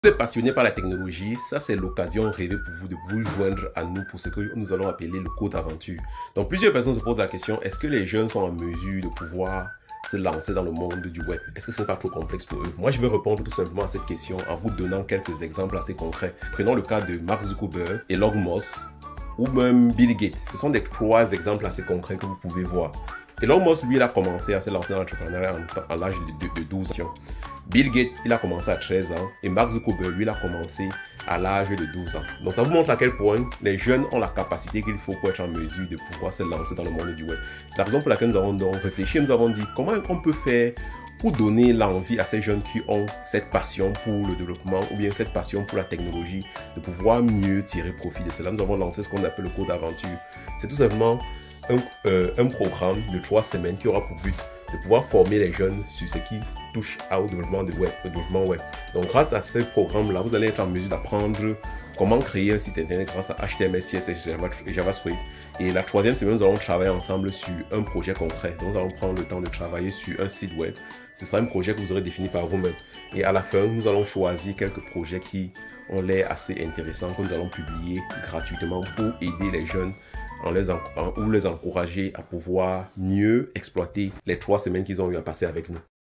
Développement web pour les enfants de 11 à 15 ans | Code Aventure
Nous avons le plaisir de lancer un Code Aventure de 3 semaines pour les enfants de 11 à 15 ans (du 17 juillet au 04 août 2023).

« Code Aventure» est un Camp d'entraînement passionnant et enrichissant de trois semaines qui contribuera à lancer les jeunes de 11 à 15 ans au développement web.

Lors DE ce programme, les participants apprendront les bases du HTML, CSS, JavaScript et WordPress, des compétences essentielles pour créer leurs propres sites web.

Ensuite, Les apprentis développeurs web travailleront sur un thème WordPress existant pour créer leur propre site web.

À la troisième semaine du programme, nous allons sélectionner les projets les plus créatifs que nous hébergerons sur le web pour encourager et valoriser le travail de nos jeunes codeurs.